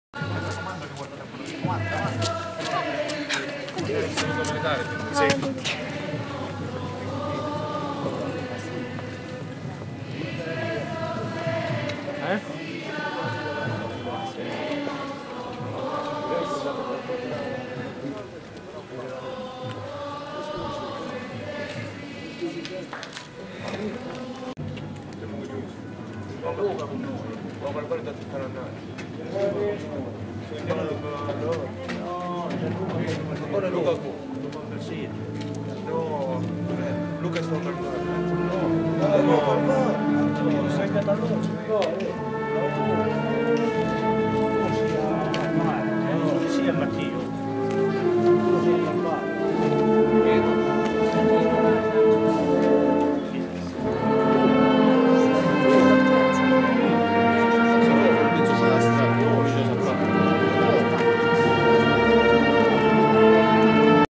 Processione dell'Addolorata Parte 6 - Pizzo Calabro - La Pasqua del 2012
Il breve video riprende il percorso più al buio di tutto il tragitto e cioè il tratto di Via Marcello Salomone. Continuo è l'accompagnamento con canti e musiche della Banda Musicale.